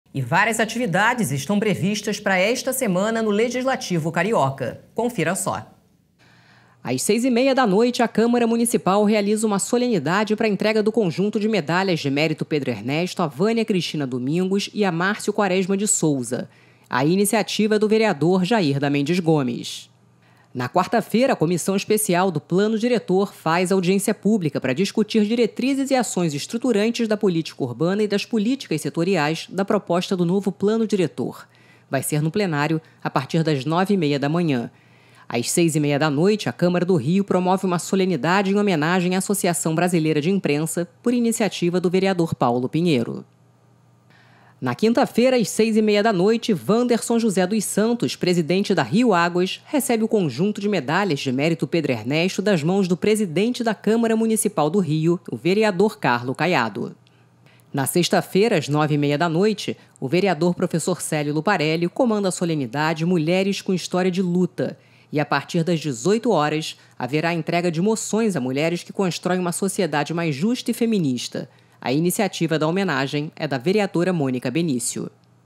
Agenda da Semana: confira as atividades previstas na Câmara do Rio
Confira, na reportagem completa, a agenda legislativa para os dias 28 a 31 de março.